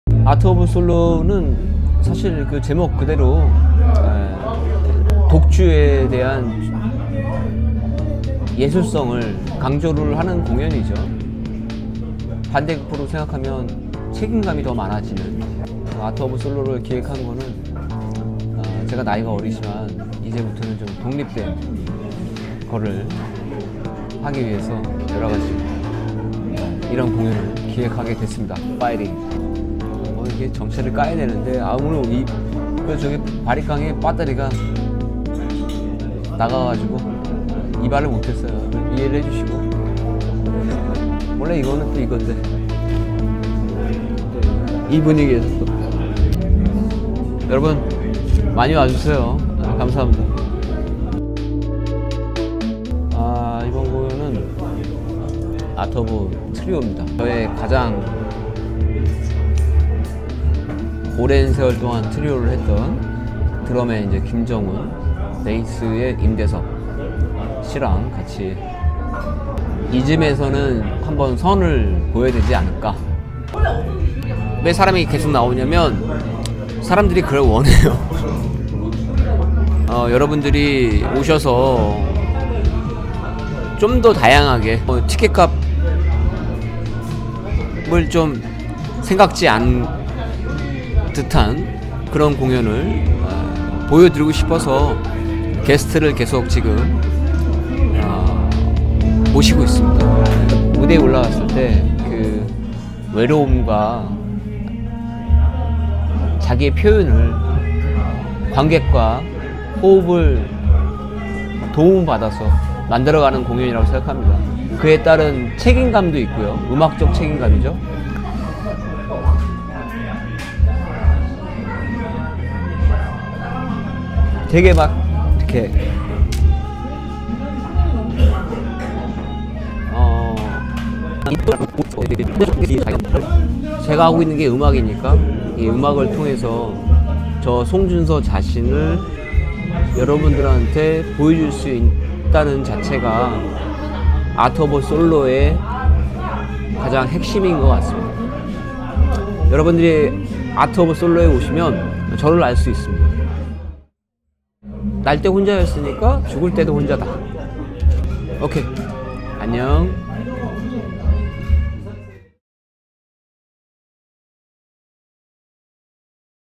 [월간공연] 재즈피아니스트 송준서 인터뷰 (5월 공연은 5/29 토 6시 재즈트리오~!)
춤(𝐃𝐚𝐧𝐜𝐞) I 5/29 sat. 6PM | 송준서트리오

한 달에 한 번씩 해온 공연이 어느덧 7번째입니다.
일년이 12달이니 비로소 반은 왔다는 생각이 드네요-😌
일곱 번째 공연을 준비하며 월간공연에 대해 간단한 인터뷰를 해보았습니다.

매 공연을 준비하는 마음은 다 새롭지만
이번 달 공연은 제 2막을 연다는 생각으로더욱 비장하게 댐비려하고있어요..!

그래서!! 비장하게!! 5월은!!
언제나 최고의 합과 시너지를 냈던 송준서 트리오의 공연을 준비했습니다.
"아..이게 재즈지!" 라는 강렬한 에너지를 느끼실 수 있을거예요.

춤 (𝐃𝐚𝐧𝐜𝐞) I 𝐓𝐇𝐄 𝐀𝐑𝐓 𝐎𝐅 𝐓𝐑𝐈𝐎

공연관련 정보는 인스타그램에서 가장 빠르게 만나보실 수 있어요.

⚡️5/29(토) 6PM 교대역 더그란데뮤직스튜디오
⚡️Piano  송준서 I Bass 임대섭 I Drums 김정훈
⚡️예매: 인터파크 티켓 (프로필 링크)